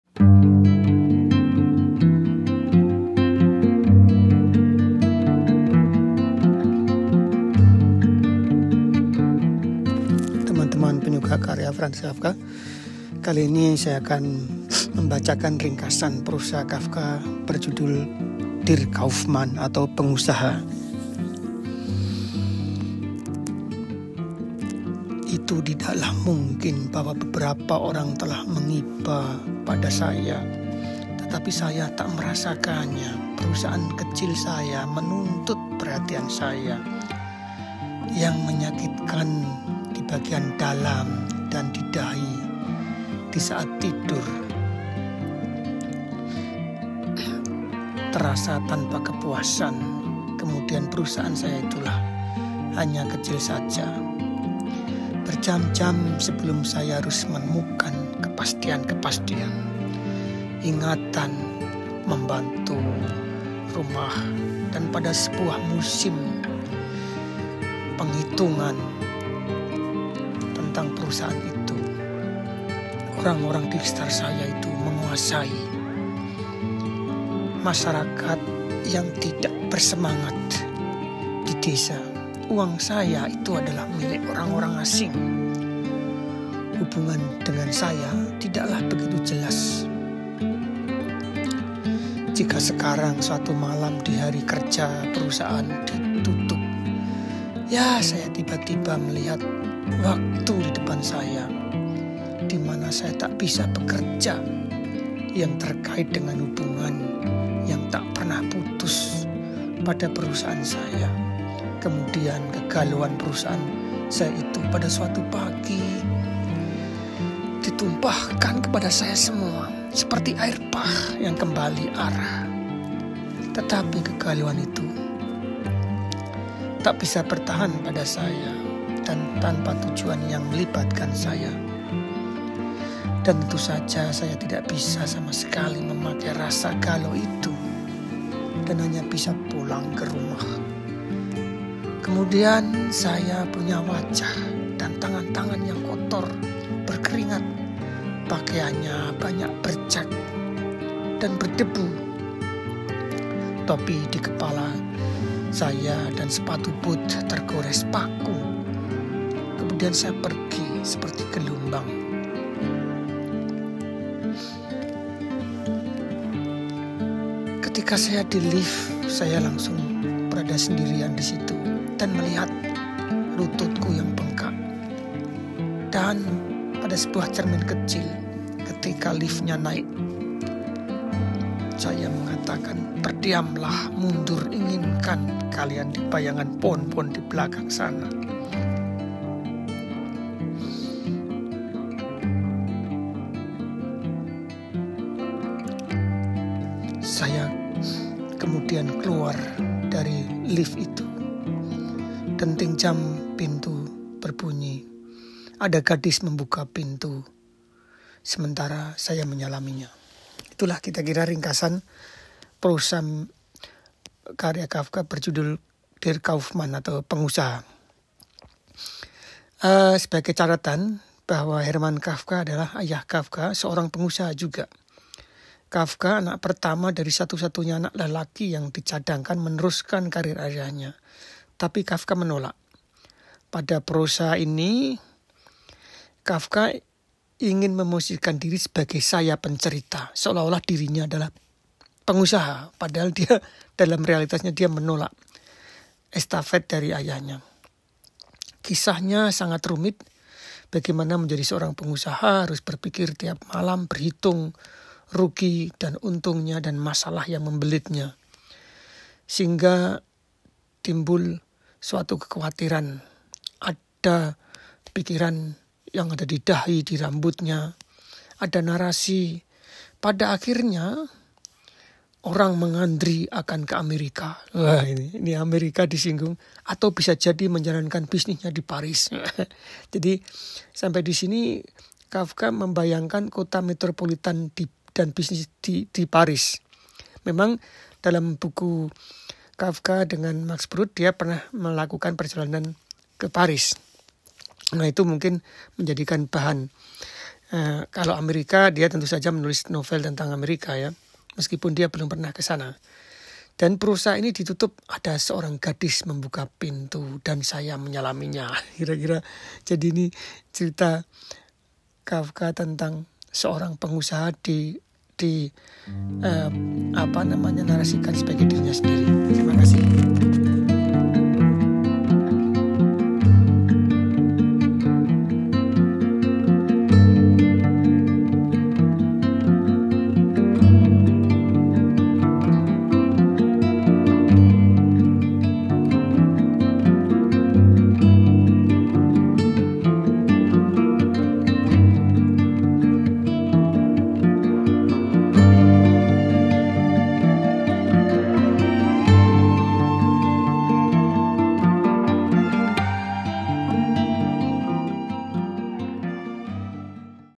Sang Pengusaha : Prosa Mini Franz Kafka
Salah satu prosa mini Franz Kafka, berjudul Der Kaufmann, Sang Pengusaha,  meskipun Kafka sendiri enggan menjadi pengusaha sebagaimana diharapkan keluarganya.